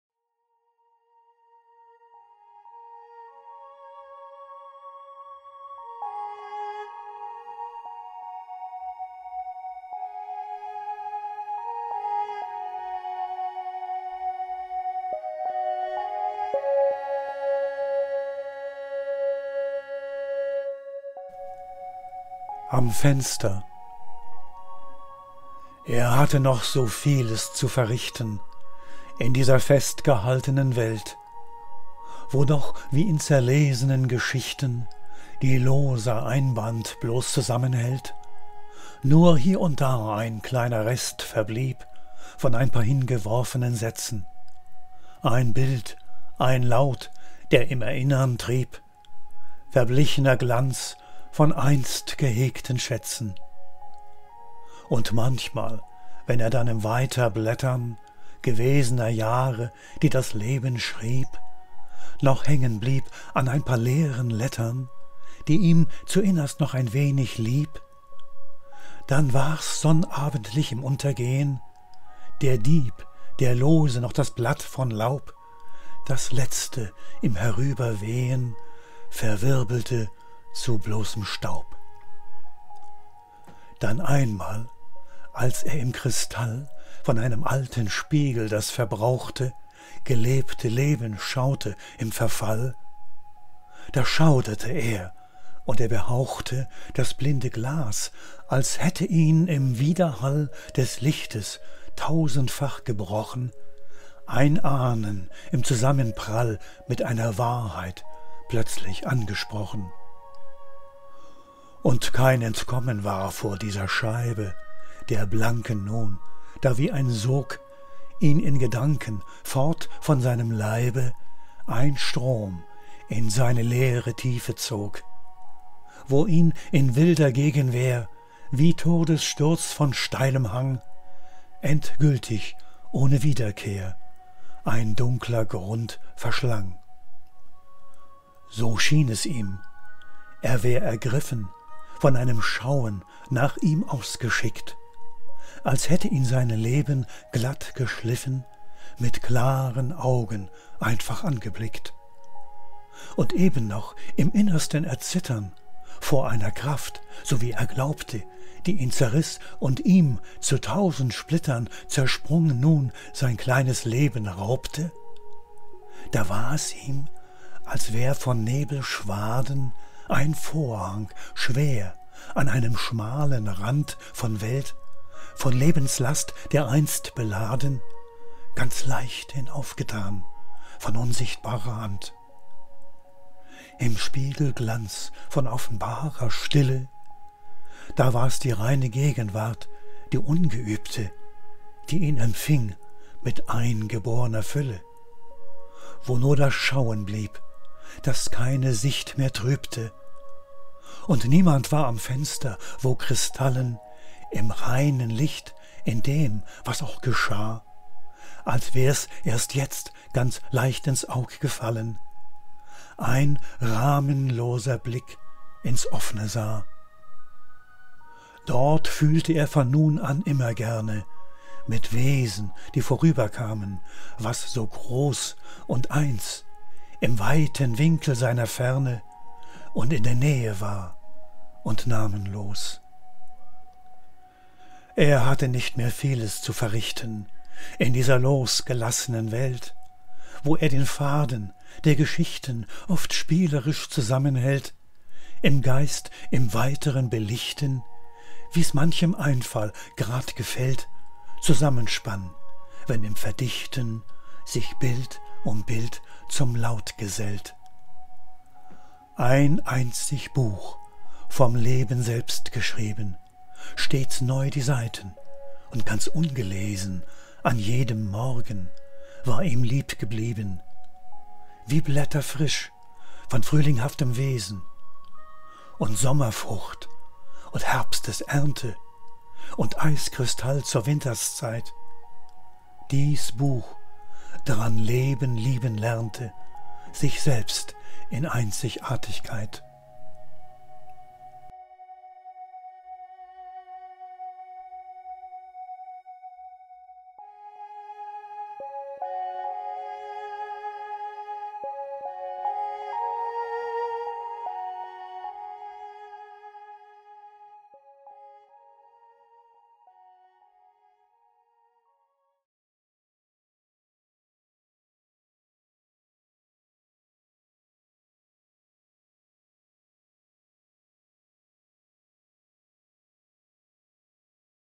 Michael Hozzel: Am Fenster
Aus dem Lyrik-Band „Fluss ohne Ufer“
Pomaska-Brand-Verlag, 2014
Bildquelle:Pinterest

Am Fenster

Er hatte noch so vieles zu verrichten
in dieser festgehaltenen Welt,
wo doch wie in zerlesenen Geschichten,
die loser Einband bloß zusammenhält,
nur hier und da ein kleiner Rest verblieb
von ein paar hingeworfenen Sätzen:
Ein Bild, ein Laut, der im Erinnern trieb,
verblichener Glanz von einst gehegten Schätzen

Und manchmal, wenn er dann im Weiterblättern
gewesener Jahre, die das Leben schrieb,
noch hängen blieb an ein paar leeren Lettern,
die ihm zuinnerst noch ein wenig lieb,
dann war’s, sonnabendlich im Untergehen,
der Dieb, der lose noch das Blatt von Laub,
das letzte, im Herüberwehen
verwirbelte zu bloßem Staub

Dann einmal, als er im Kristall
von einem alten Spiegel das verbrauchte,
gelebte Leben schaute im Verfall,
da schauderte er, und er behauchte
das blinde Glas, als hätte ihn im Widerhall
des Lichtes, tausendfach gebrochen,
ein Ahnen im Zusammenprall
mit einer Wahrheit plötzlich angesprochen

Und kein Entkommen war vor dieser Scheibe,
der blanken nun, da wie ein Sog
ihn in Gedanken, fort von seinem Leibe,
ein Strom in seine leere Tiefe zog,
wo ihn in wilder Gegenwehr
wie Todessturz von steilem Hang
endgültig ohne Wiederkehr
ein dunkler Grund verschlang

So schien es ihm, er wär ergriffen
von einem Schauen, nach ihm ausgeschickt,
als hätte ihn sein Leben, glatt geschliffen,
mit klaren Augen einfach angeblickt.
Und eben noch im innersten Erzittern
vor einer Kraft, so wie er glaubte,
die ihn zerriss und ihm, zu tausend Splittern
zersprungen, sein kleines Leben raubte,

da war es ihm, als wär von Nebelschwaden
ein Vorhang, schwer, an einem schmalen Rand
von Welt, von Lebens Last dereinst beladen,
ganz leichthin aufgetan von unsichtbarer Hand.
Im Spiegelglanz von offenbarer Stille,
da war’s die reine Gegenwart, die ungeübte,
die ihn empfing mit eingeborener Fülle,
wo nur das Schauen blieb, das keine Sicht mehr trübte

Und niemand war am  Fenster, wo kristallen
im reinen Licht in dem, was auch geschah,
als wär’s erst jetzt ins Aug gefallen,
ein rahmenloser Blick ins Offene sah.
Dort fühlte er von nun an immer gerne
mit Wesen, die vorüberkamen, was so  groß
und eins im weiten Winkel seiner Ferne
und in der Nähe war und namenlos

Er hatte nicht mehr vieles zu verrichten
in dieser losgelassenen Welt,
wo er den Faden, der Geschichten
oft spielerisch zusammenhält,
im Geist im weiteren Belichten,
wie’s manchem Einfall grad gefällt,
zusammen spann, wenn im Verdichten
sich Bild um Bild zum Laut gesellt:

Ein einzig Buch, vom Leben selbst geschrieben,
stets neu die Seiten und ganz ungelesen
an jedem Morgen, war ihm lieb geblieben
wie Blätter frisch, von frühlingshaftem Wesen,
und Sommerfrucht und Herbstes Ernte
und Eiskristall zur Winterszeit,
dies Buch, dran Leben lieben lernte
sich selbst in Einzigartigkeit

Michael Hozzel